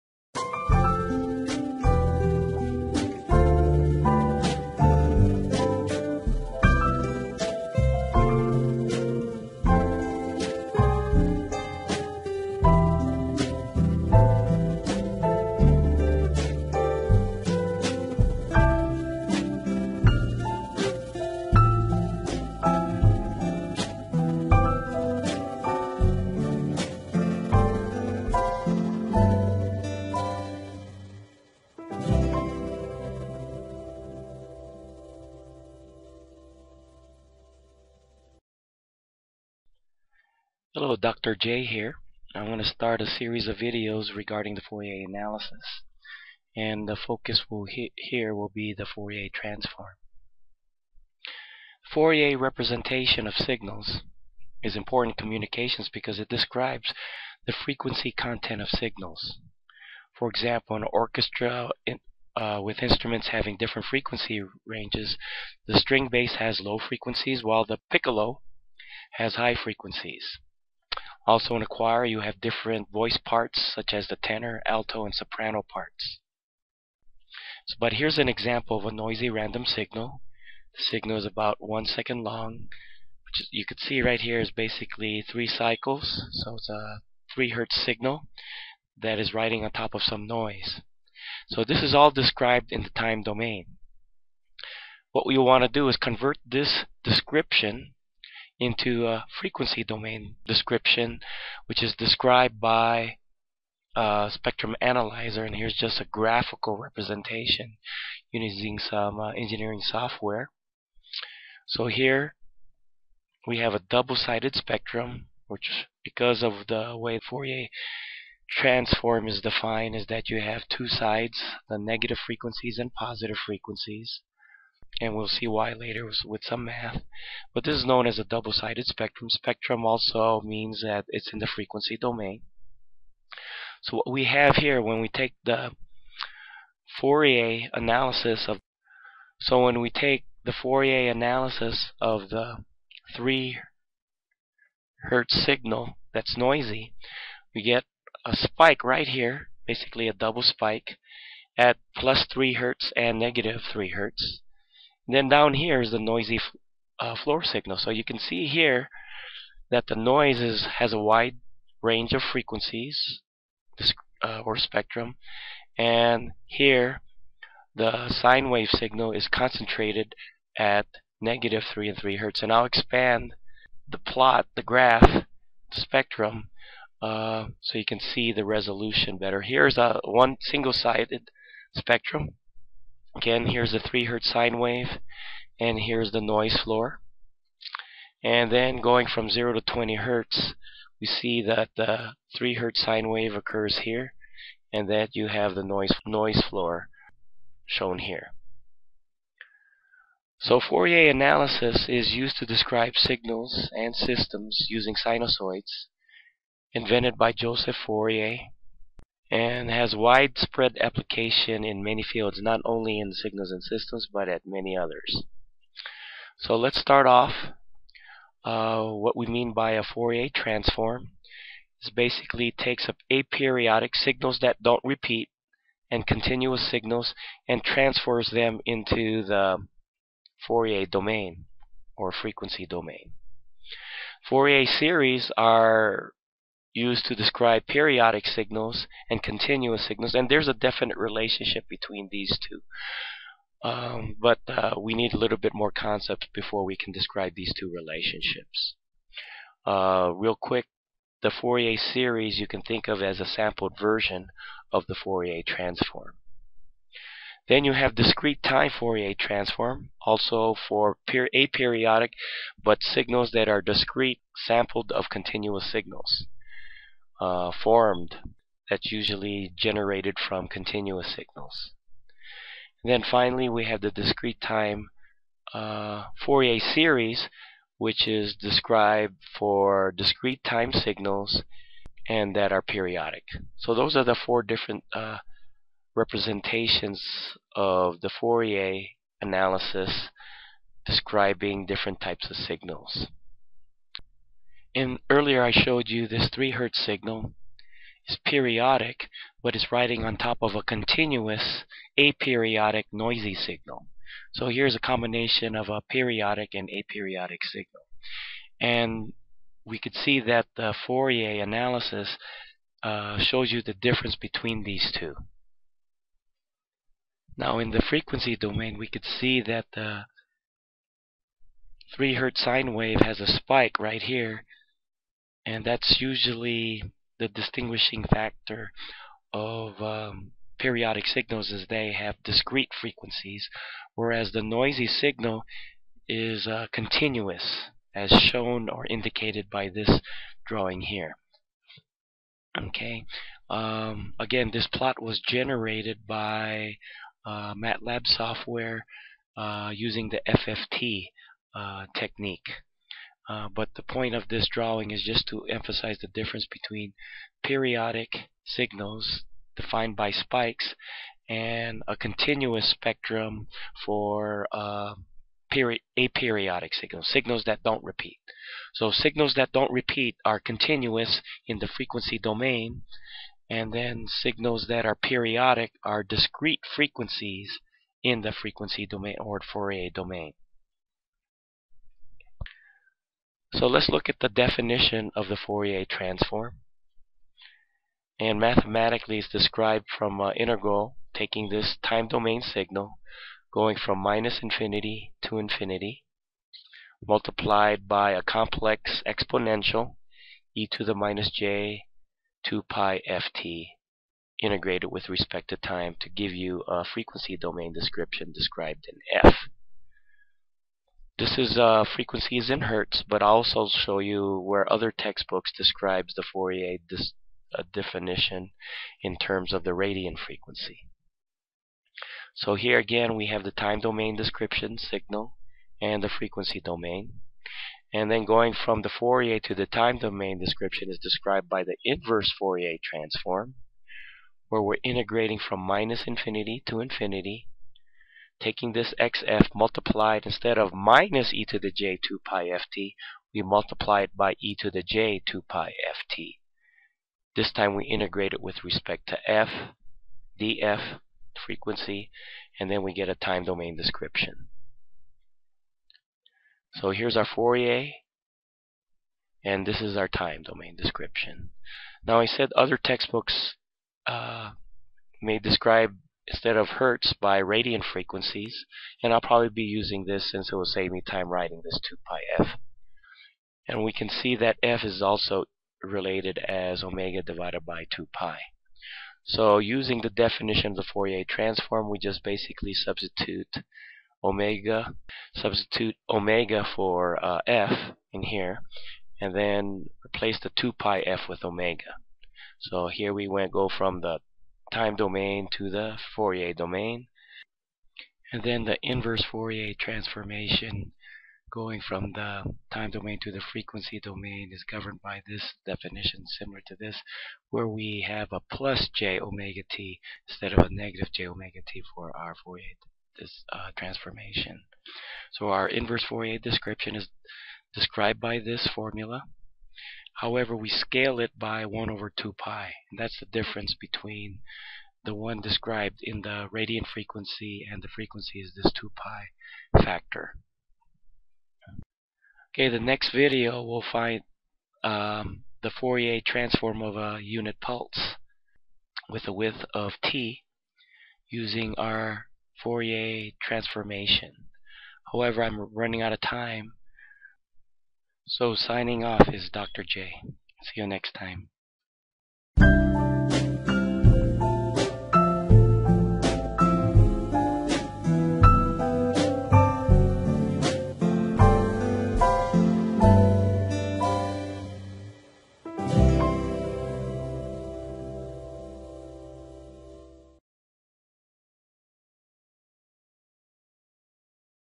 [Fourier Transform Tutorial] Introduction to Fourier Analysis (Part 1)
First of a series of videos on Fourier Analysis.   Four Fourier representations are given with initial emphasis on the Fourier Transform.